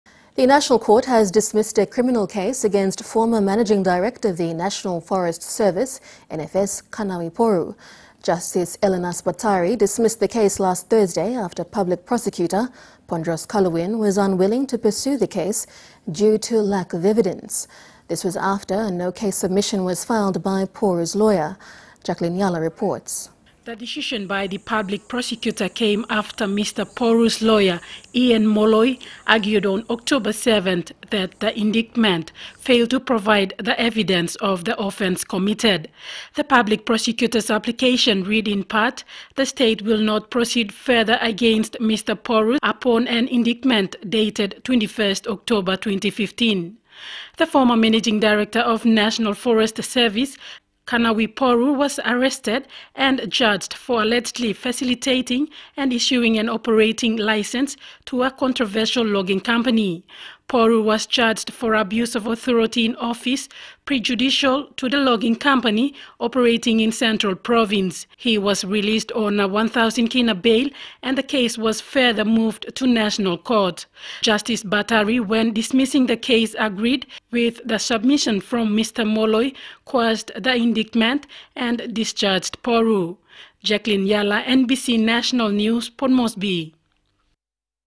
NBC PNG News - NFS Boss case dismissed
Justice Ellenas Batari dismissed the case last Thursday after public prosecutor Pondros Kaluwin was unwilling to pursue the case due to lack of evidence